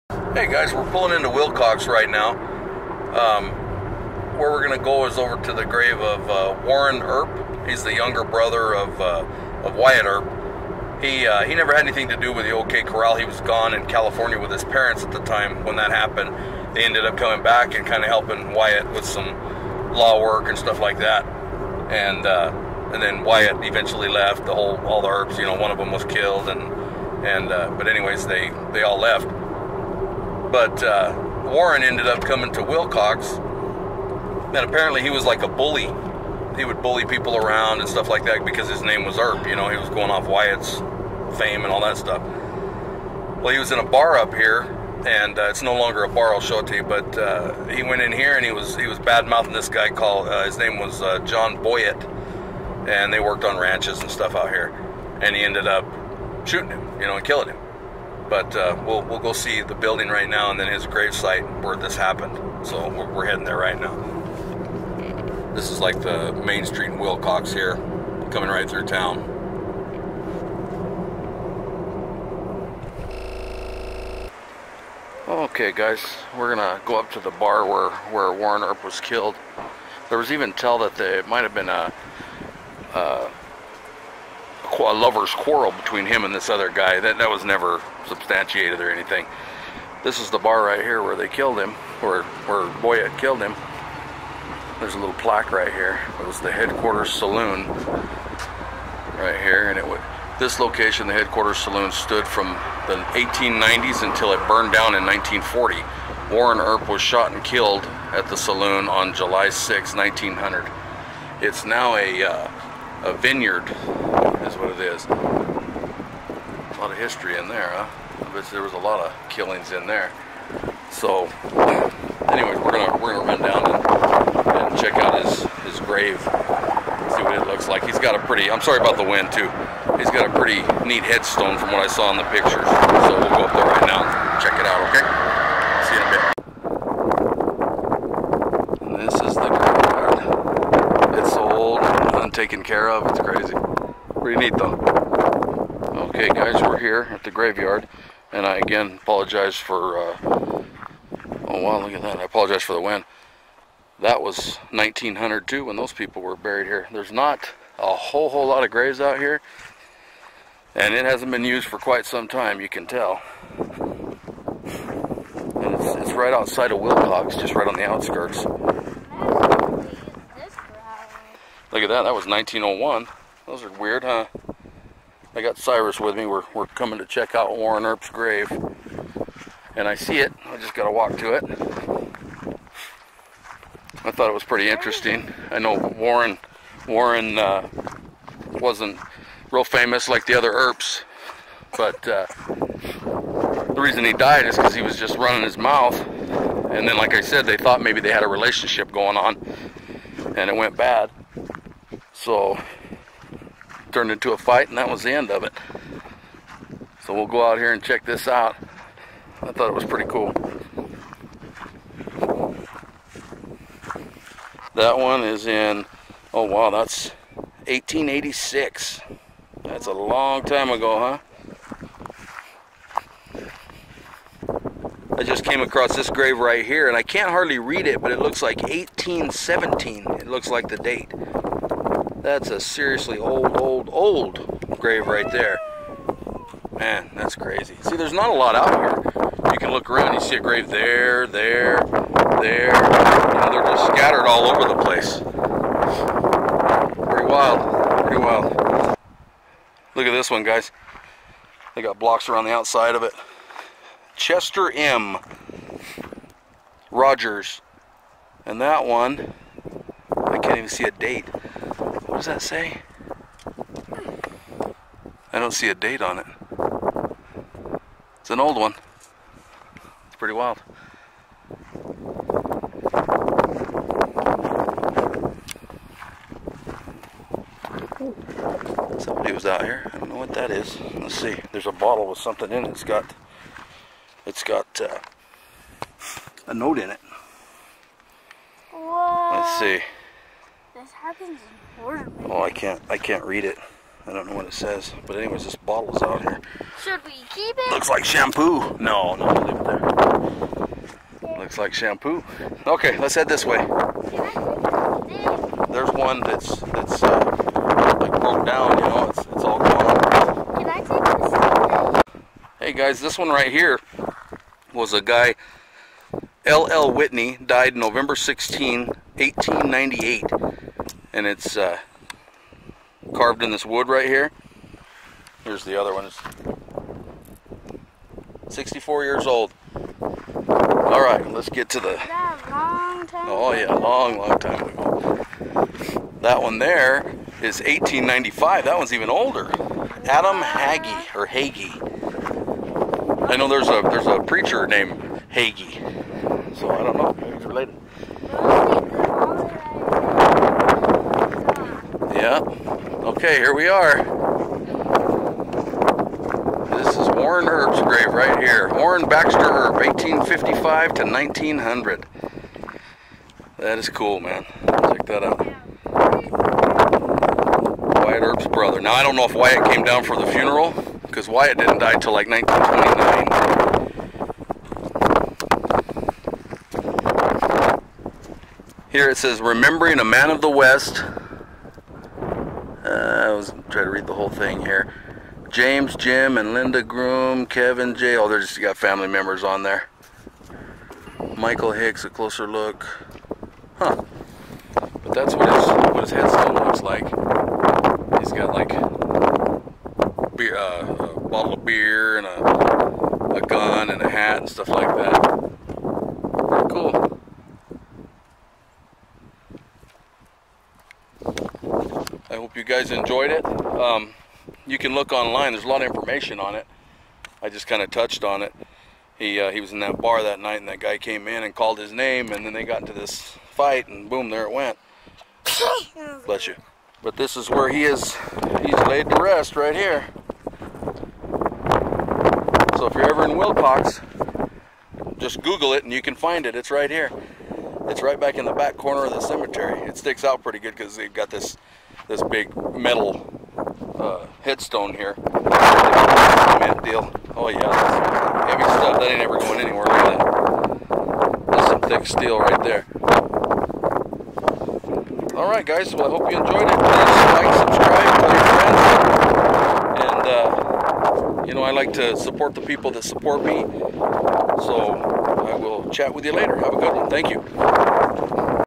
Wyatt Earps Family. Warren Earps Arizona Grave
Let's go and check out the grave of Wyatt Earps younger brother Warren Earp. He was killed in Willcox Arizona in a bar fight. I hope you enjoy the video.